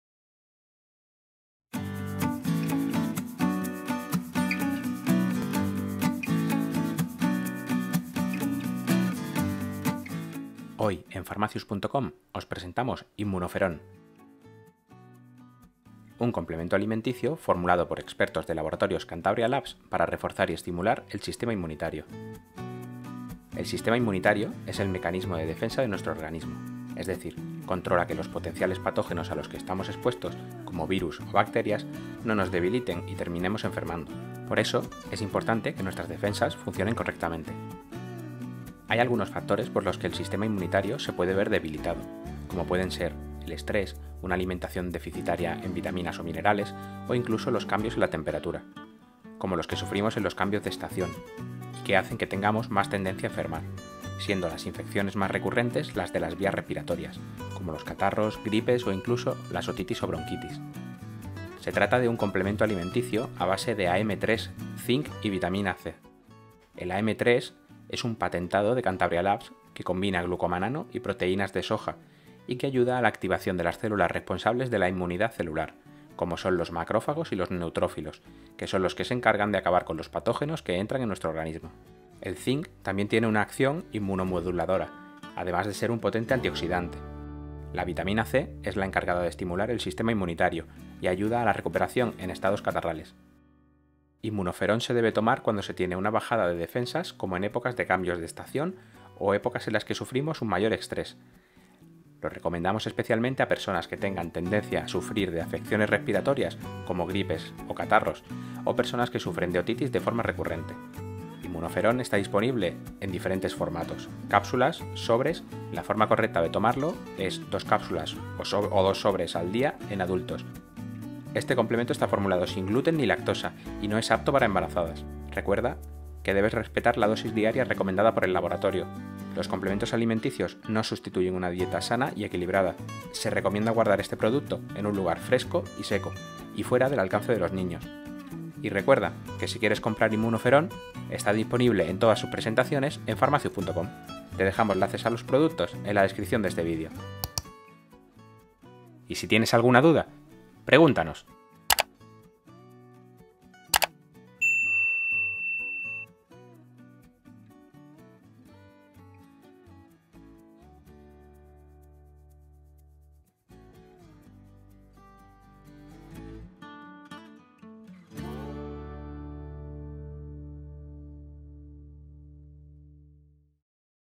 Inmunoferon Sobres o Cápsulas. Cómo Tomar
INMUNOFERON SOBRES O CÁPSULAS de Cantabria Labs ►

A continuación comentamos Qué es, Para qué sirve, Cómo se toma, Composición, Funciona, Posología, Contraindicaciones.

Comparte tus opiniones sobre el complementos de Inmunoferon Sobres o Inmunoferon Cápsulas en los comentarios.

TRANSCRIPCIÓN:

Hoy en pharmacius.com os presentamos inmunoferon

un complemento alimenticio formulado por expertos de laboratorios Cantabria Labs para reforzar y estimular el sistema inmunitario.

El sistema inmunitario es el mecanismo de defensa de nuestro organismo, es decir, controla que los potenciales patógenos a los que estamos expuestos, como virus o bacterias, no nos debiliten y terminemos enfermando, por eso, tener en correcto funcionamiento de nuestras defensas.

Hay algunos factores por los que el sistema inmunitario se puede ver debilitado, como pueden ser: el estrés, una alimentación deficitaria en vitaminas o minerales o incluso los cambios en la temperatura, como las que sufrimos en los cambios de estación y que hacen que tengamos más tendencia a enfermar, siendo las infecciones más recurrentes las de vías respiratorias, como los catarros, gripes o incluso las otitis o bronquitis.

Se trata de un complemento alimenticio a base de AM3, zinc y Vitamina C.
En AM3 es un patentado de Cantabria labs que combina glucomanano y proteínas de soja y que ayuda a la activación de las células responsables de la inmunidad celular, como son los macrófagos y los  neutrófilos, que son las que se encargan de acabar con los patógenos que entran en nuestro organismo.

El Zinc también tiene acción inmunomoduladora  además de ser un potente antioxidante.
Vitamina C, es la encargada de estimular el sistema inmunitario y que ayuda a la recuperación en estados catarrales.

Se debe tomar cuando se tiene una bajada de defensas como en épocas de cambio de estación o épocas de estrés. Lo recomendamos especialmente a personas que tengan tendencias a sufrir de afecciones respiratorias como gripes o catarros o personas que sufren de otitis de forma recurrente.
Inmunoferon está disponible en diferentes formatos, cápsulas, sobres y el formato junior en jarabe.

La forma correcta de tomarlo es dos cápsulas o sobres al dia en adultos y una cápsula o sobre al díal en niños.

- Están formulados sin gluten ni lactosa, y no es apto para embarazadas.

- Es importante respetar la dosis diaria recomendada por el laboratorio.
- Y debes saber que los complementos alimenticios no son sustitutivos de una dieta variada y equilibrada.
- Se recomienda guardar en lugar fresco y seco y mantener fuera del alcance de los niños.

Recuerda que si quieres comprar Inmunoferon está disponible en Pharmacius.com, te dejamos los enlaces a loa productos en la descripción de este video.

Y si tienes alguna duda, pregúntanos!